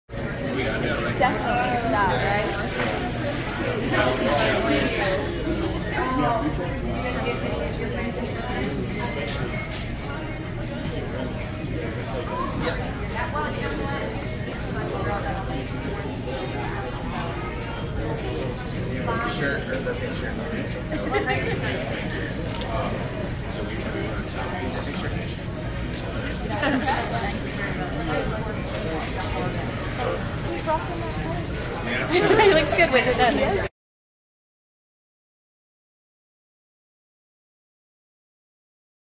Malik Monk Signing
Malik Monk signs autographs for U.K. fans at Allsports in Fayette Mall.